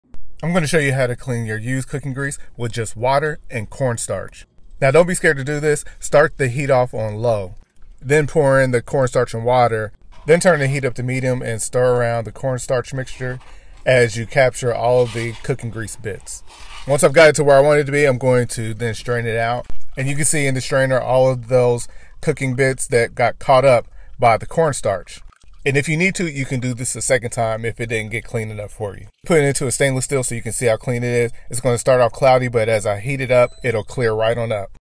Clean Used Cooking Oil With Water and Corn Starch
Cleaning used grease or oil is easy by simply using water and corn starch!